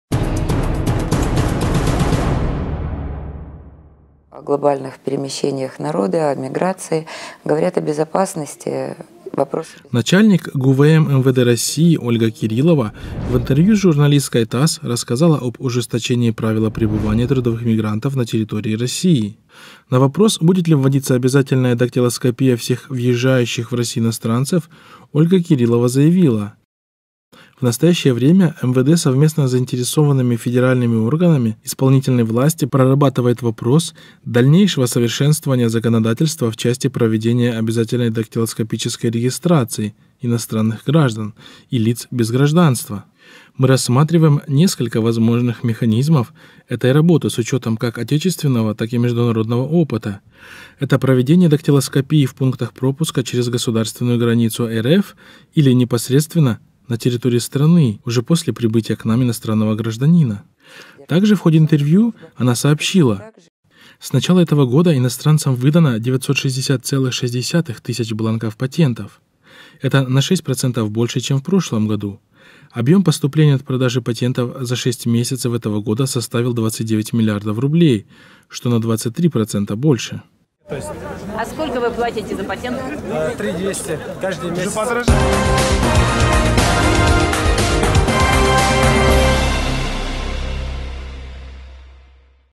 Трудовые мигранты пополнили российский бюджет на 29 млрд
Начальник ГУВМ МВД России Ольга Кириллова в интервью с журналисткой ТАСС рассказала об ужесточении правила пребывания трудовых мигрантов на территории России.